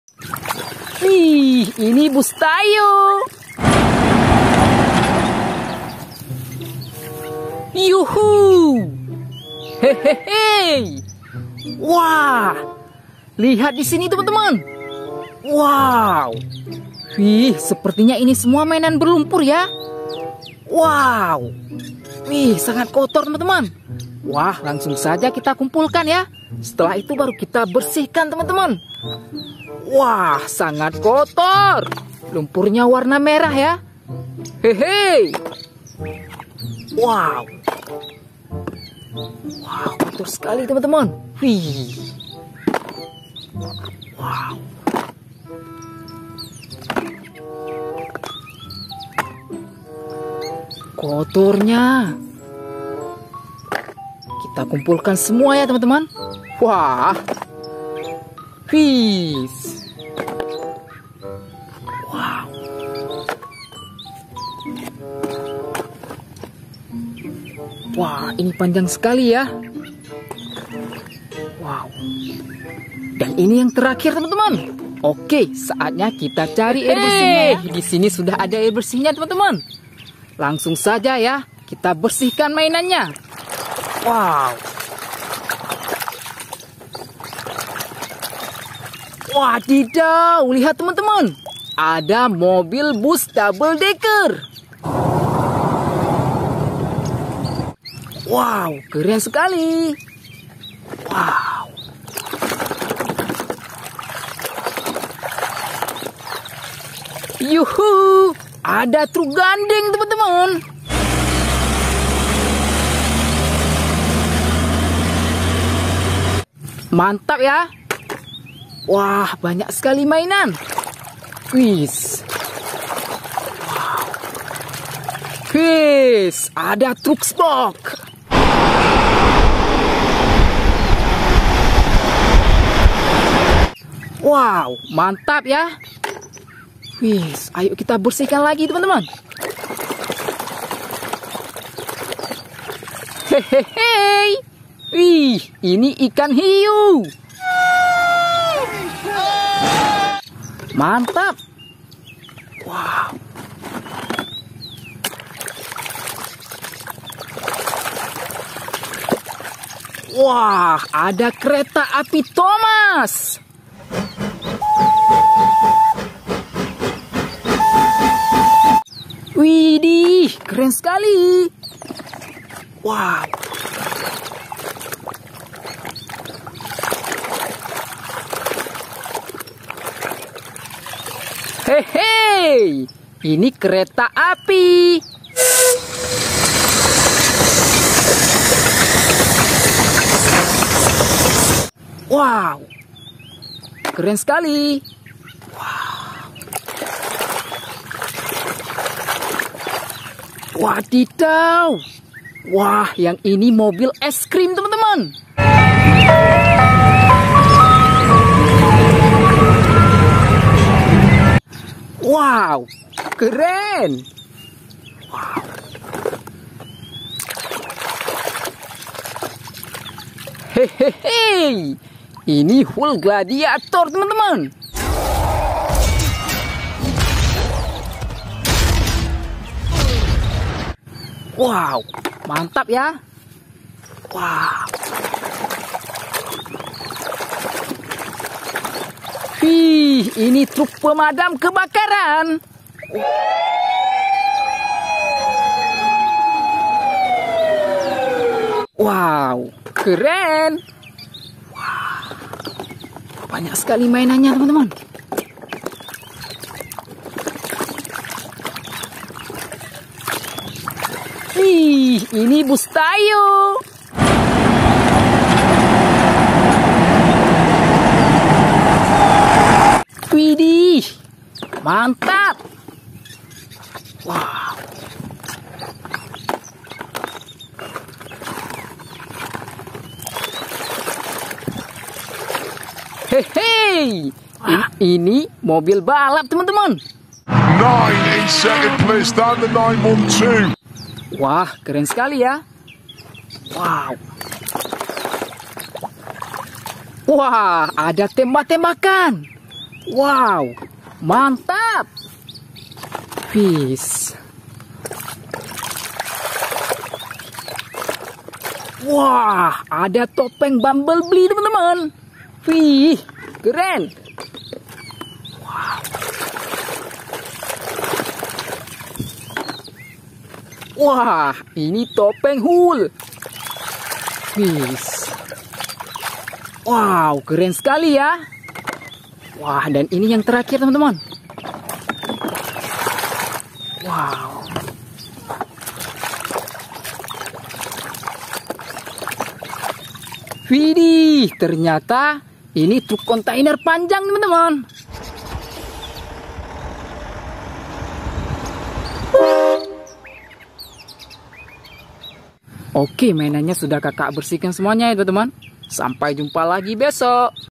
Membersihkan Mainan Mobil Mobilan, Mobil Balap, Kereta Api, Bus Tayo, Truk Pemadam Kebakaran, Truk
Membersihkan Mainan Mobil Mobilan, Mobil Balap, Kereta Api, Bus Tayo, Truk Pemadam Kebakaran, Truk Box